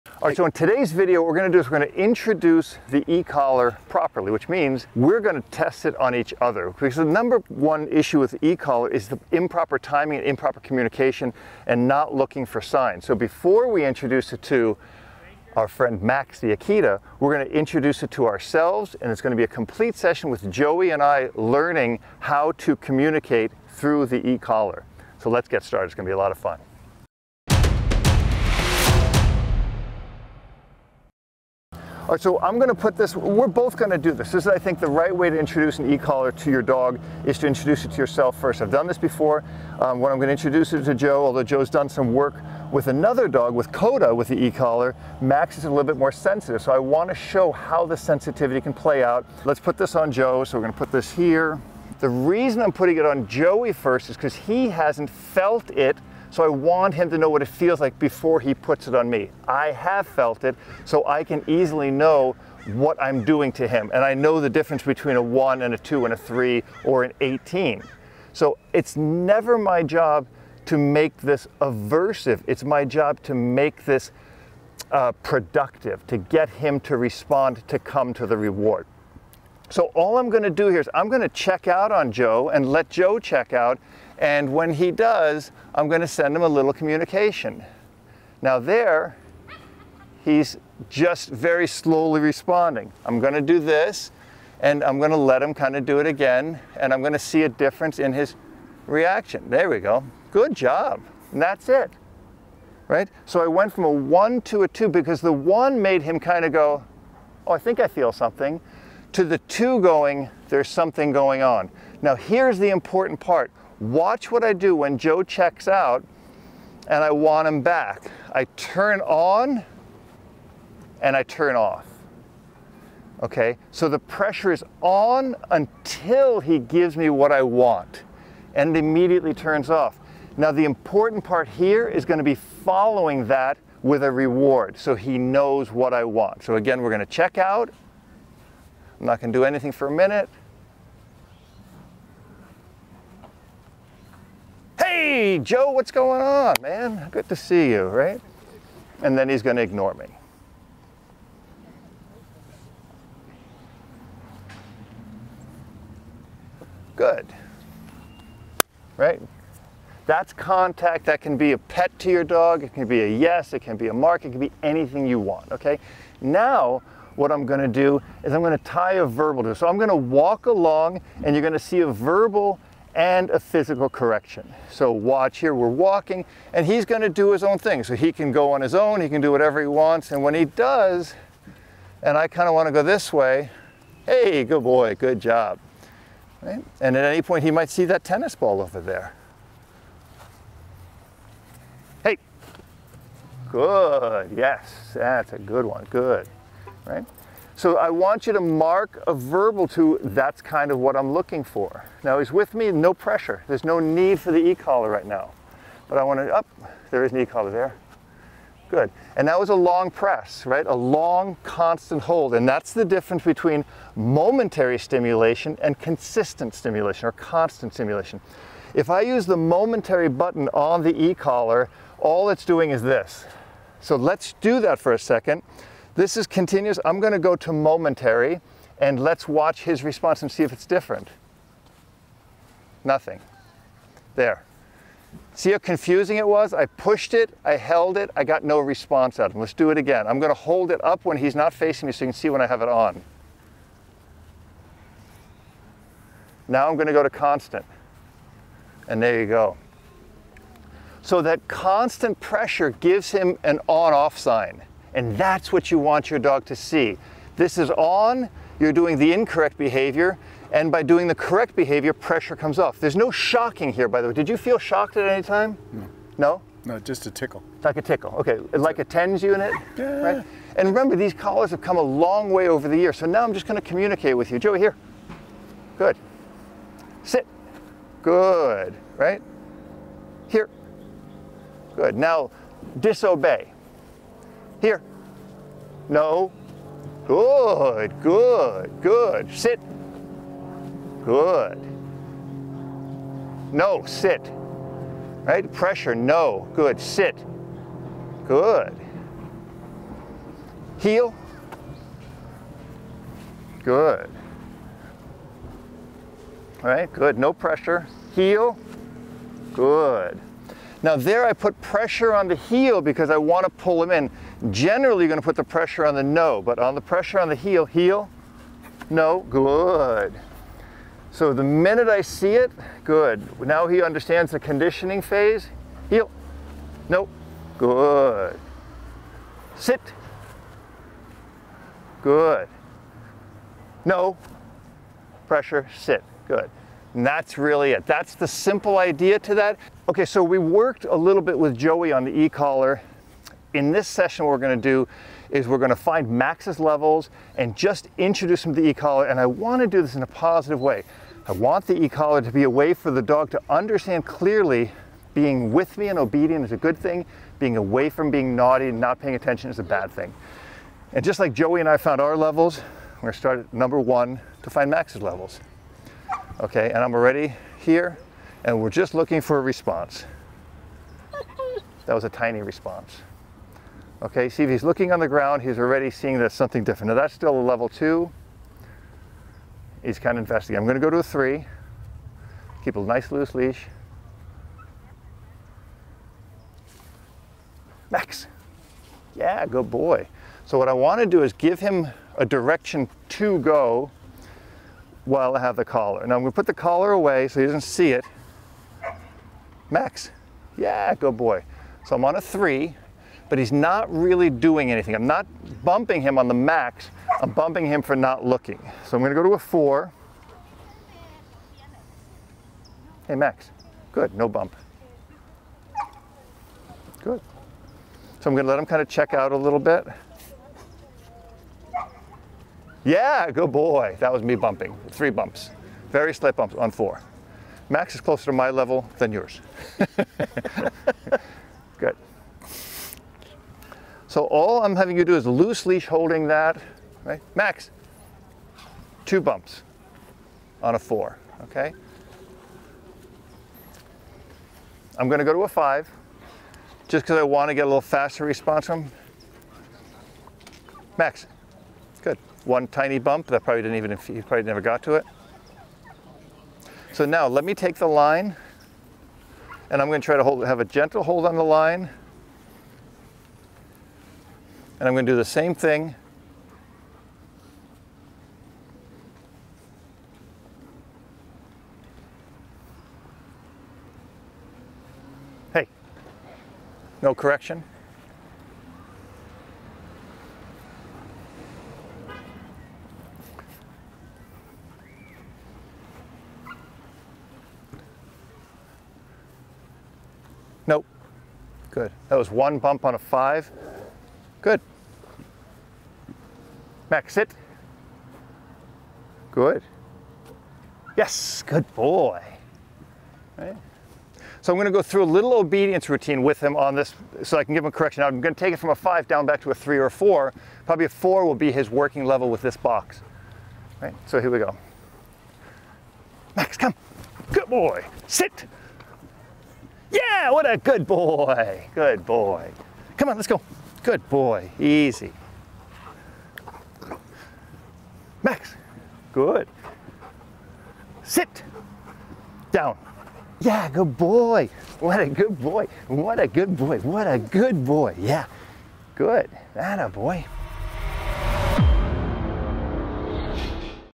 Are Ecollars SAFE for Your DOG?
In this video lesson I show Joey how to learn the ecollar and then how to introduce it to his Akita named Max.  We spend a significant amount of time understanding what it will feel like, how to use the stimulation settings and what to expect from our dog.

I think this is critical to understand and make the tool fair for your dog.  The ecollar can be one of the best training tools when used properly.

There are several factors that can help the ecollar fit better and function more precisely.  That involves the contact points properly connecting on your, or your dogs neck.  In this video I use a very effective tool, The Martin System Chameleon Extender.  This can be used with just about any ecollar you may have.